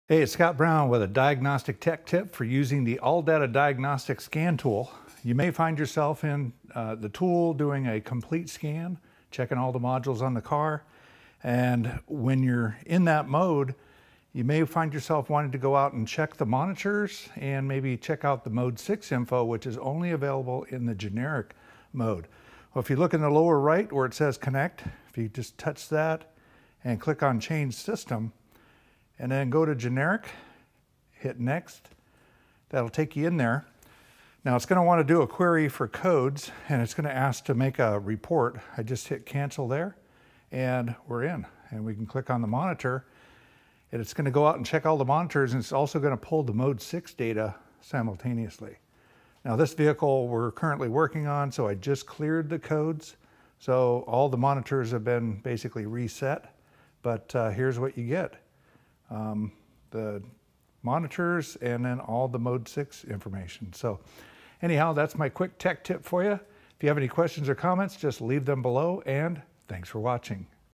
Tech Tip: ALLDATA Diagnostics scan tool
In this video, Scott Brown, ASE Master Certified L1 Technician, provides tech tips on how to use the ALLDATA Diagnostics scan tool.